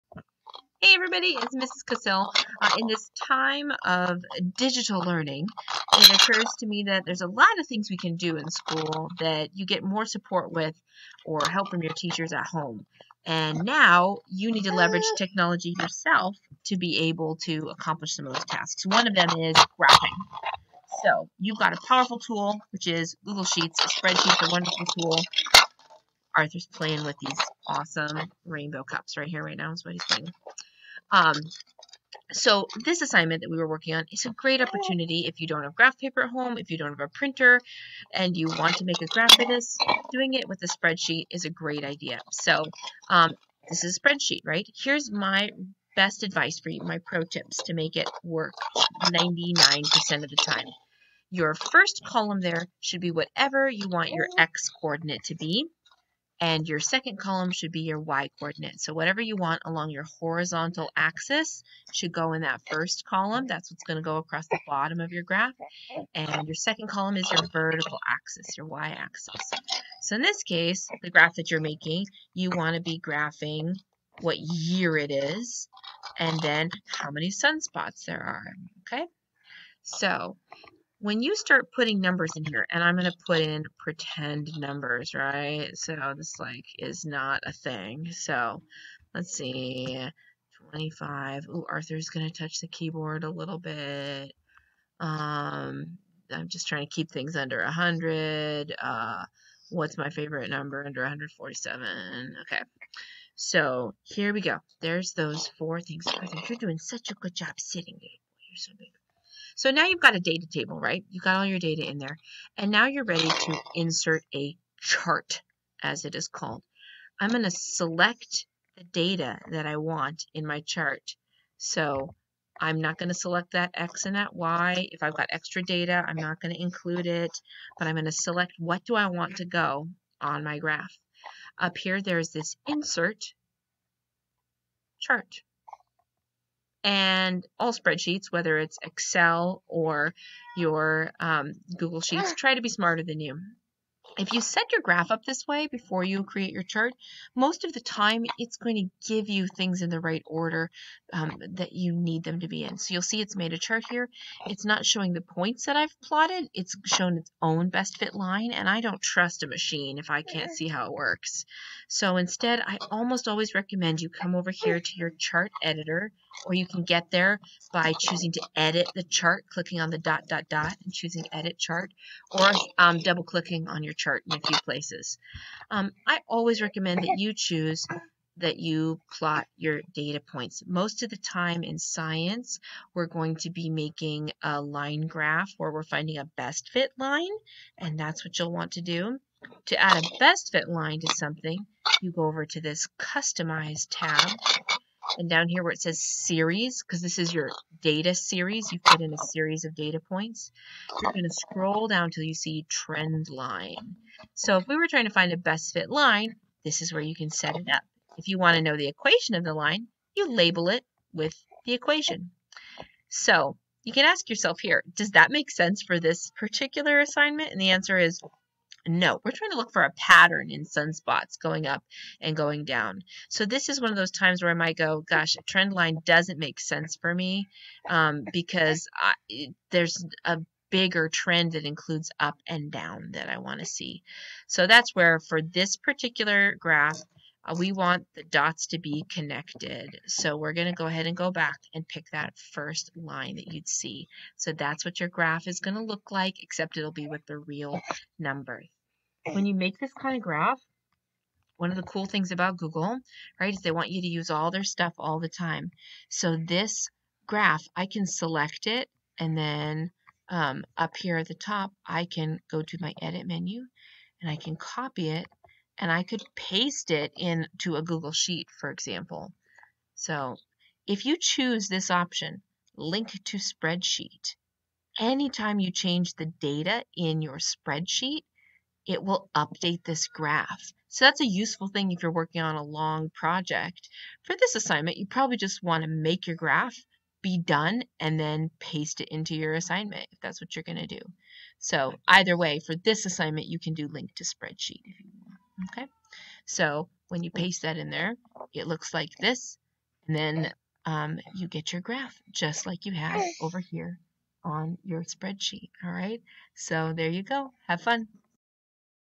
Students: How to graph with Google Sheets
Need to make a graph on your own?  Here are my pro tips!